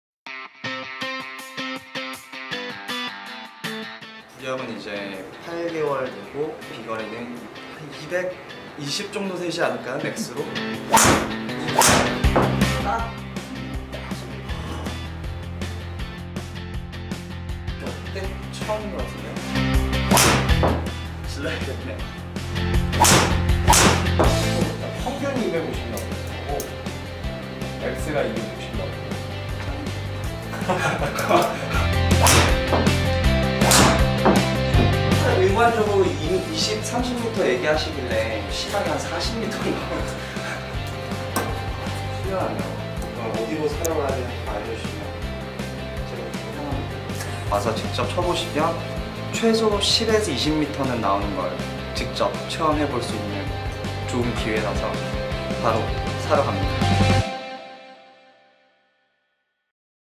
[캘러웨이] EPIC TRIAL : 아마추어 시타 #5
✔ 비거리가 40M 증가?!
✔ 지금 바로 사러 갑니다!

아마추어 골퍼가 극찬한 GBB 에픽의 비거리와 관용성.
영상으로 확인해 보세요!

시타에 참가해주신 김세현(Travis Kim)님께 감사드립니다.